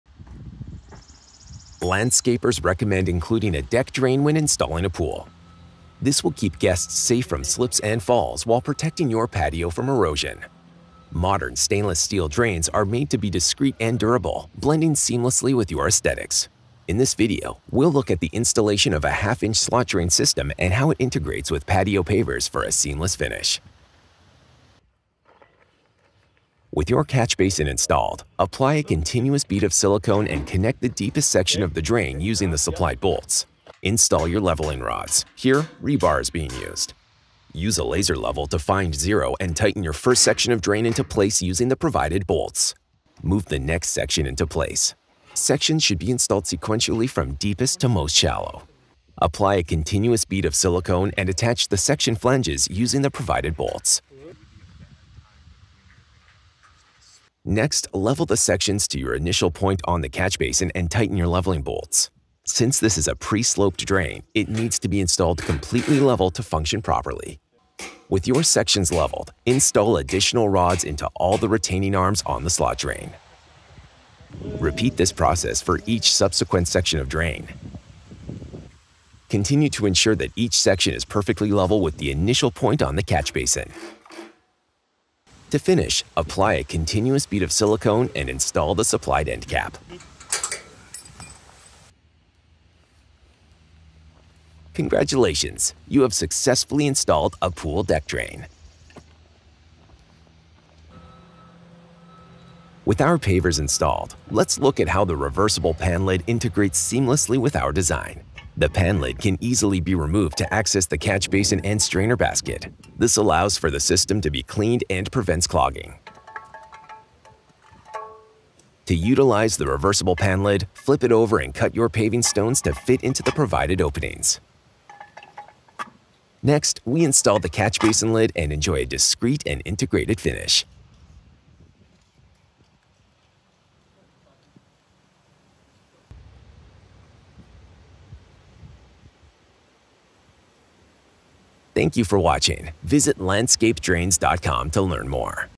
How to Install a Pool Deck Drain
Landscapers recommend including a deck drain when installing a pool. This will keep guests safe from slips and falls while protecting your patio from erosion. Modern stainless steel drains are made to be discreet and durable, blending seamlessly with your esthetics. In this video, we'll look at the installation of a half inch slot drain system and how it integrates with patio pavers for a seamless finish.

With your catch basin installed. Apply a continuous bead of silicone and connect to the deepest section of drain using the supplied bolts. Install your leveling rods. Here, rebar is being used. Use a laser level to find zero and tighten your first section of drain into place using the provided bolt. Move the next section into place. Sections should be installed sequentially from deepest to most shallow.

Apply a continuous bead of silicone and attach the section flanges using the provided bolts.

Next, level the sections to your initial point on the catch basin and tighten your leveling bolts. Since this is a pre sloped drain, it needs to be installed completely level to function properly. With your sections leveled, install additional rods into all the retaining arms on the Slot Drain.

Repeat this process for each subsequent section of drain. Continue to ensure that each section is perfectly level with the initial point on the catch basin. To finish, apply a continuous bead of silicone and install the supplied end cap.

Congratulations! You have successfully installed a pool deck drain.

With our pavers installed. Let's look at how the reversible pan lid integrates seamlessly with our design. The pan lid can easily be removed to access the catch basin and strainer basket. This allows for the system to be cleaned and prevents clogging. To utilize the reversible pan lid. Flip it over and cut your paving stones to fit into the provided openings.

Next, we install the catch basin lid and enjoy a discreet and integrated finish.